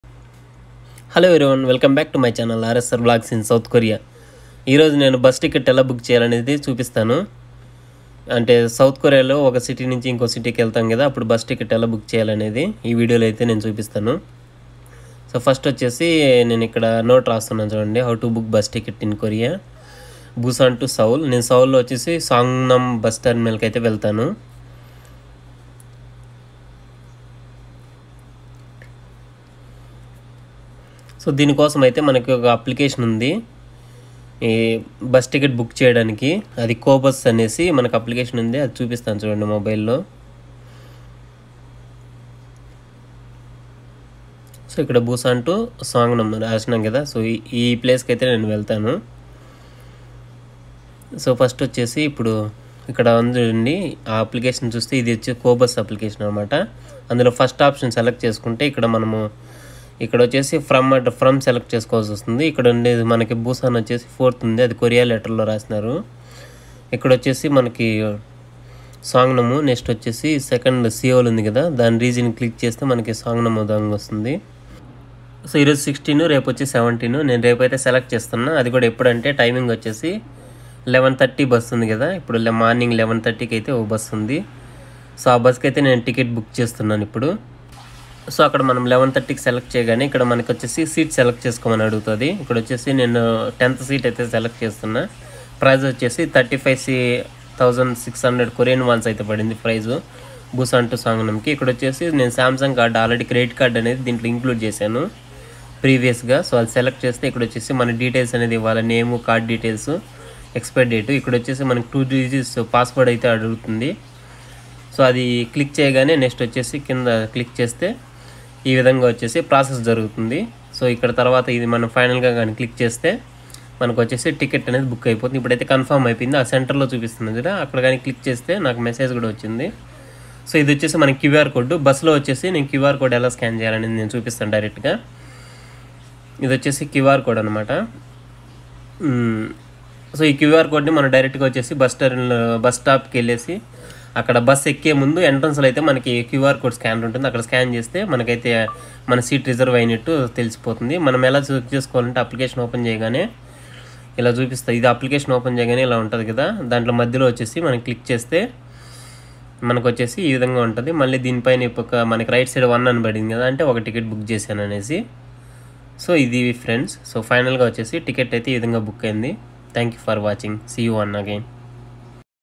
How To Book Bus Ticket in Korea ? | Busan To Seoul (Seongnam)
How to Book Bus Ticket In Korea ?

Completed visited Places